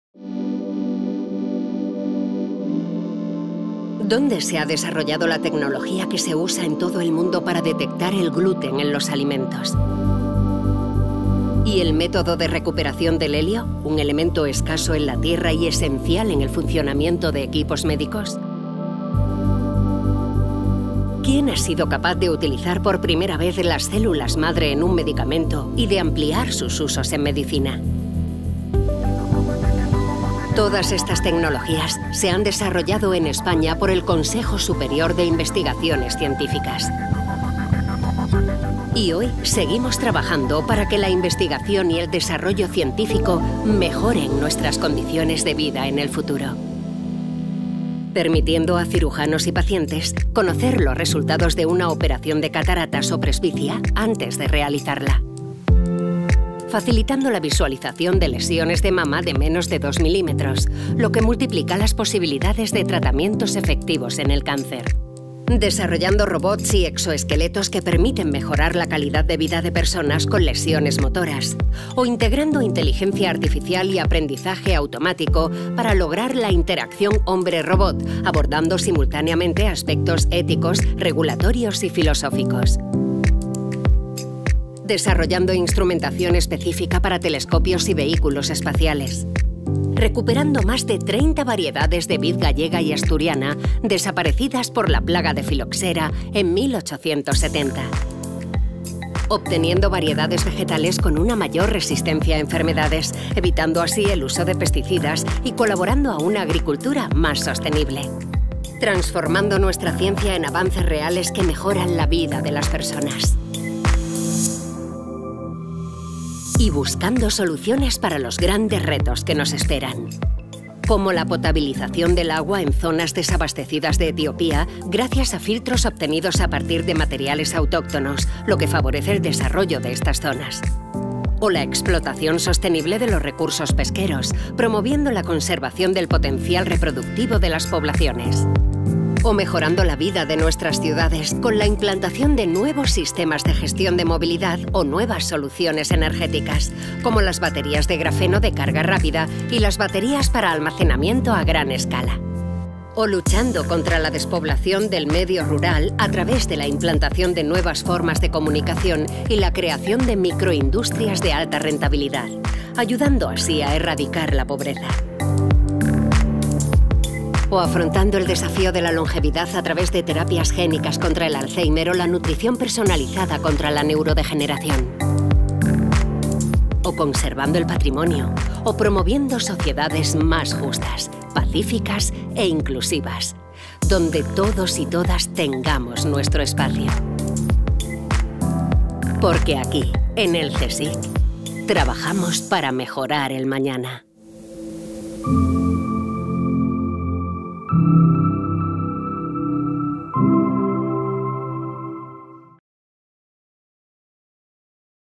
La transferencia de la tecnología en el CSIC
Repasamos algunos de los ejemplos más destacados de la transferencia de tecnología a la sociedad por parte del CSIC. Presente en campos tan diversos como la salud, la astrofísica, la energía, alimentación o apostando por una agricultura y pesca más sostenibles. Desarrollando tecnologías punteras en todo el mundo, culminando así el trasvase de conocimiento a
la sociedad. Porque en el CSIC trabajamos para mejorar el mañana.